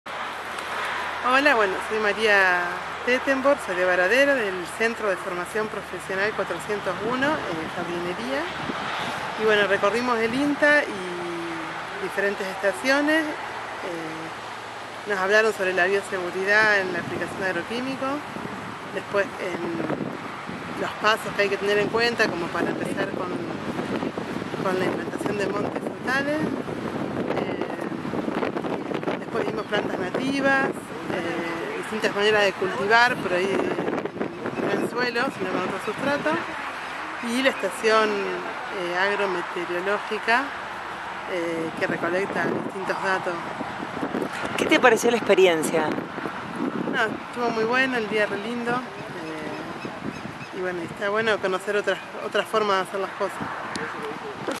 María - Centro de Formación Profesional 401 de Baradero (Jardinería)
Este es el testimonio que dejó, luego de participar de la 15ava edición de la Vidriera Tecnológica